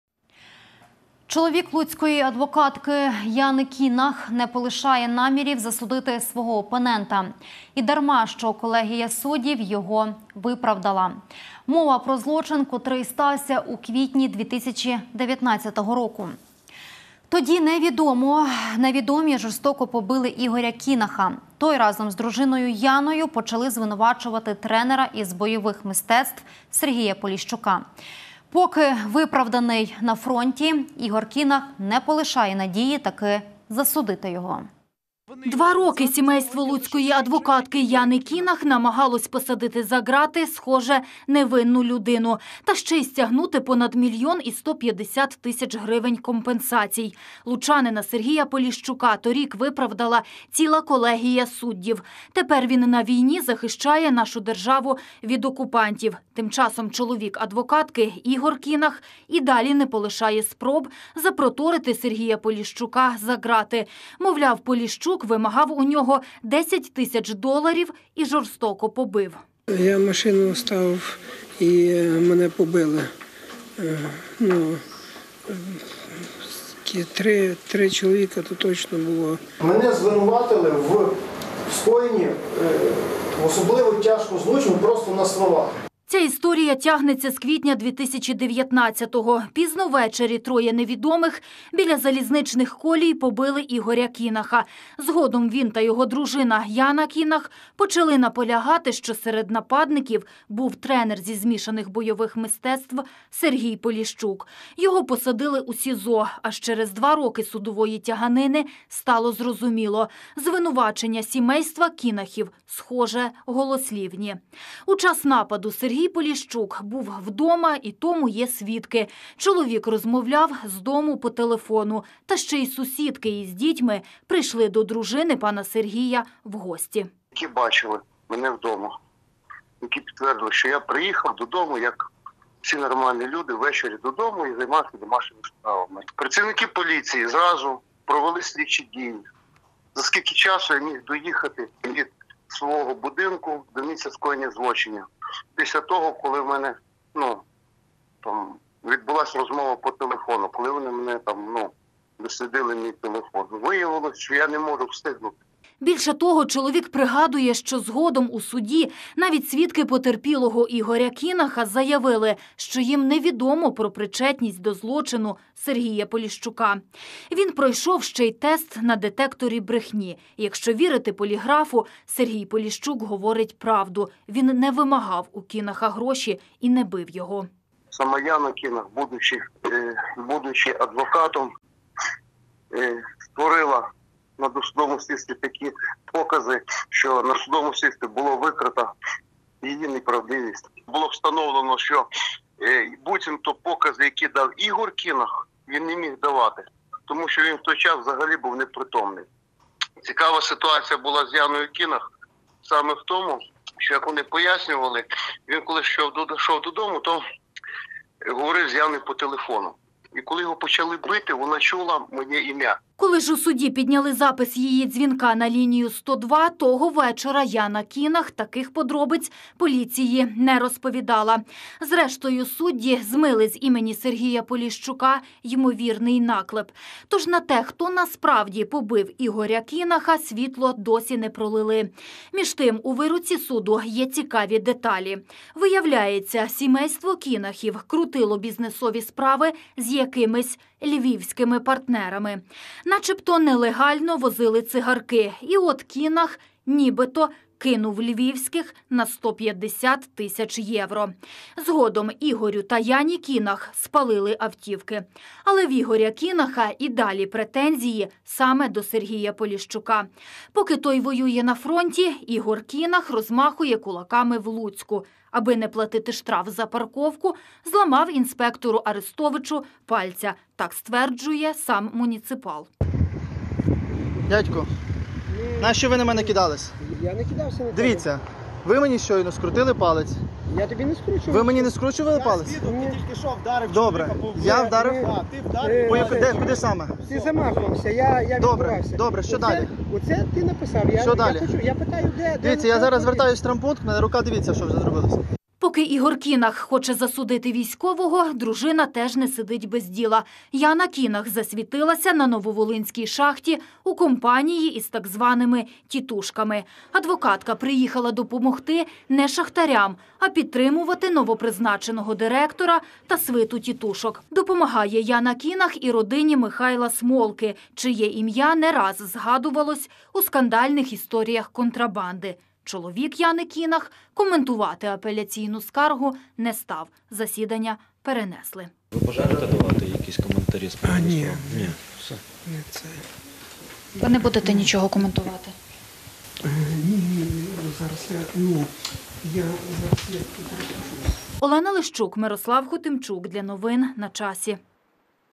Історія побиття: чоловік адвокатки Ігор Кінах не полишає спроб засудити виправданого лучанина
Чоловік луцької адвокатки Яни Кінах не полишає намірів засудити свого опонента. І дарма, що колегія суддів його виправдала. Мова про злочин, котрий стався у квітні 2019-го. Тоді невідомі жорстоко побили Ігоря Кінаха. Той разом з дружиною Яною почали звинувачувати тренера із бойових мистецтв Сергія Поліщука. Поки виправданий на фронті – Ігор Кінах не полишає надії таки засудити його.